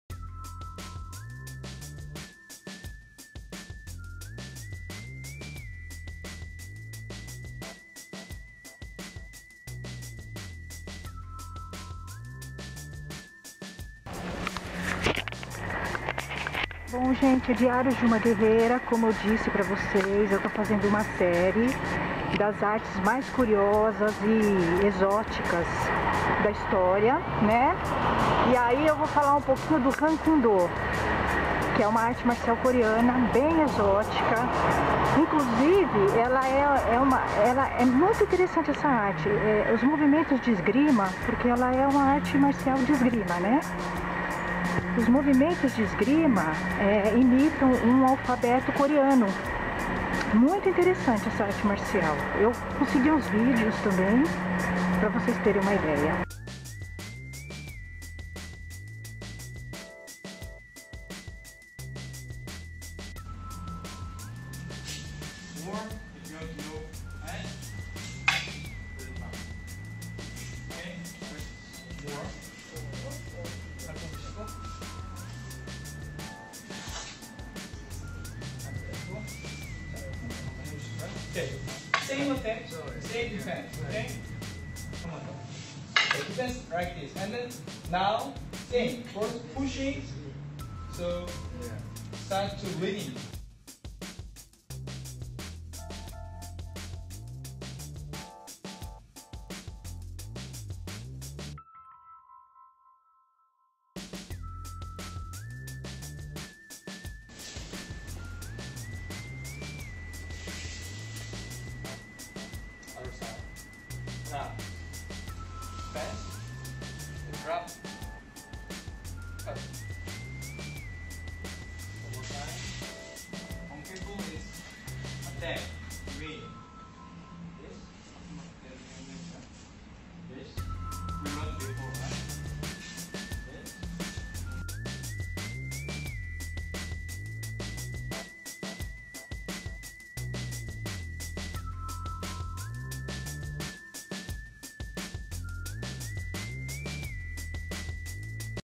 As artes marciais mais curiosas: O Hankumdo
Os diários de uma guerreira conta nesse vídeo um pouco sobre uma arte marcial coreana pouco conhecida, o Hankumdo. Foi criada pelo Grão Mestre Myung Jae-nam, fundador da International Hapkido Federation e é muito curiosa.